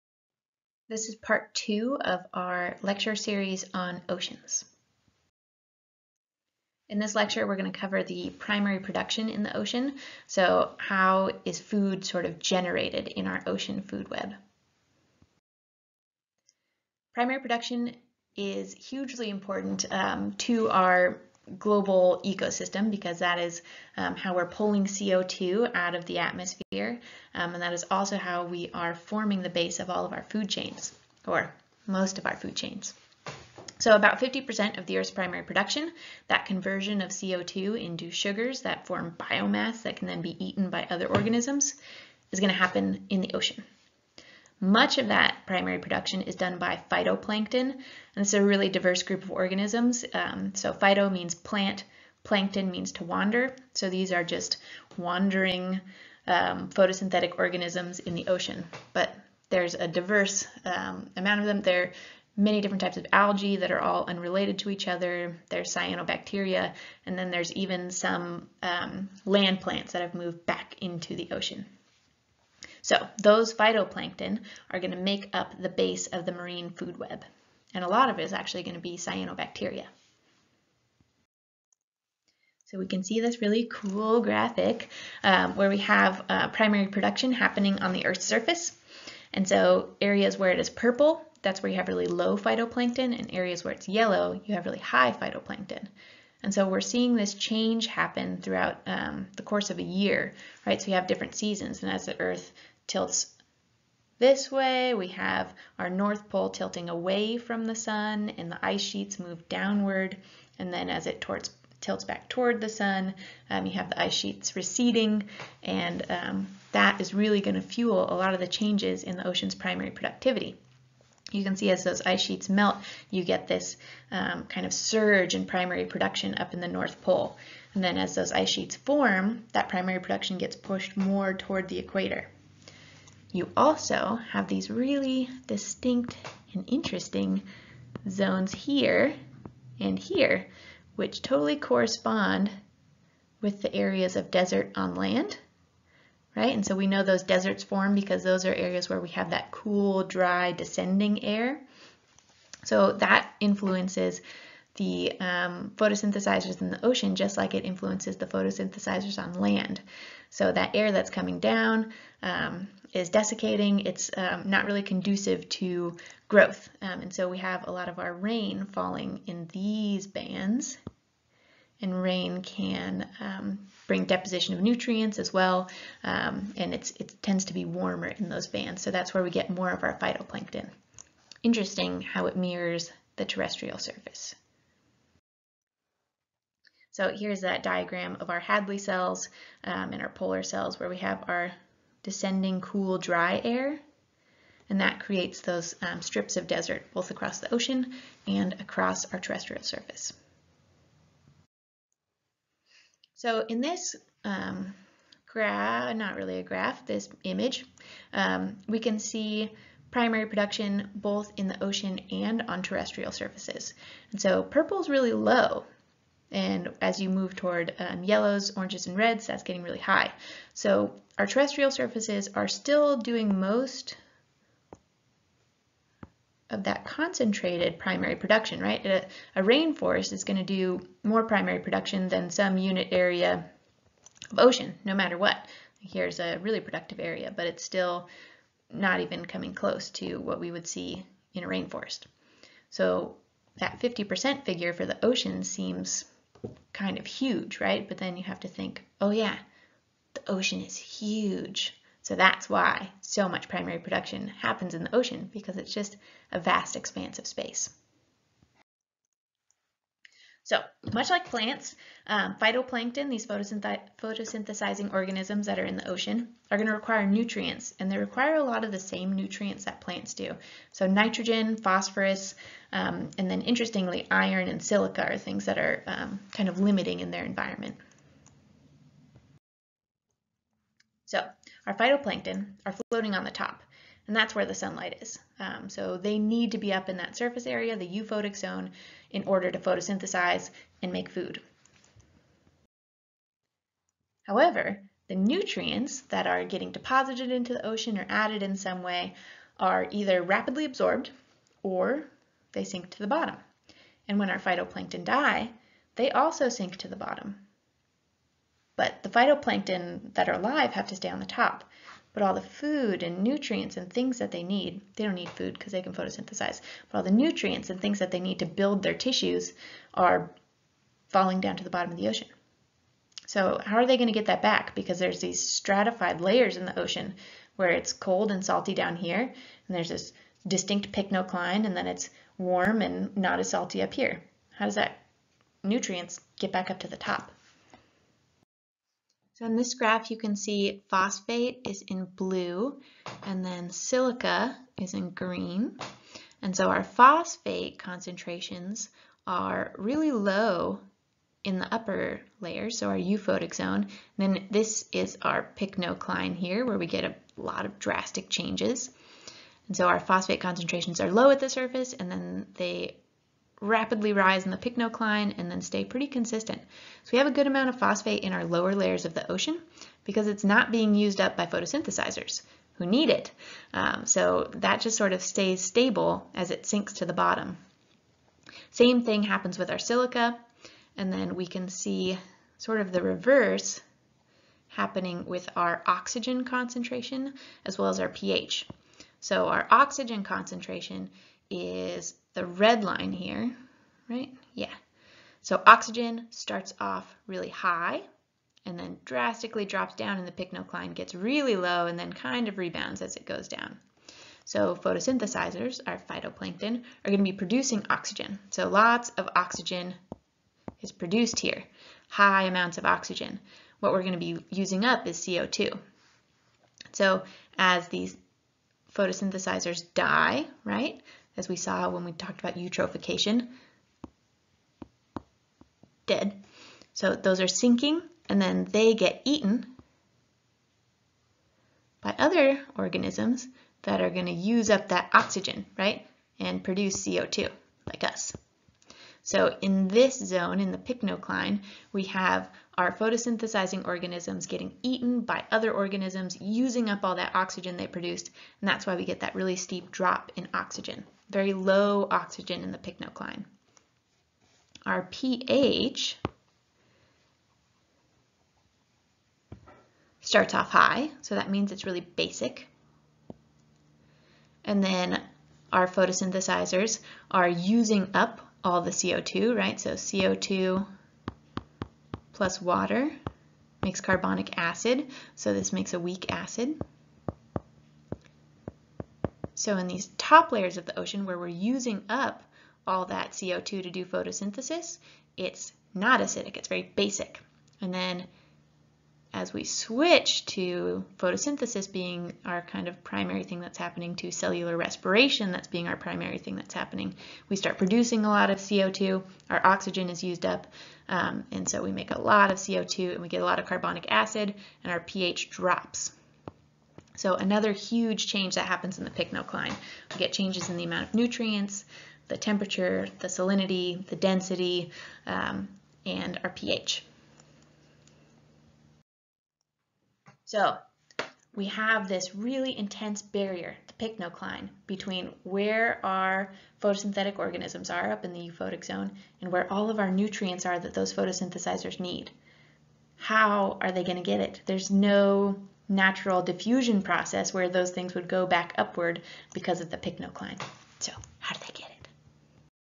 Oceans Part 2: Primary Production
After watching this video, you should be able to:
1. Define primary production and explain who is responsible for this process in the ocean biome.
2. Understand how and why dissolved oxygen, pH, and phosphate levels change with ocean depth. Consider the process of photosynthesis -- what does it use up, what does it produce, and how does this interact with water?
3. Consider how stratification of the ocean and differences in climate impact primary production.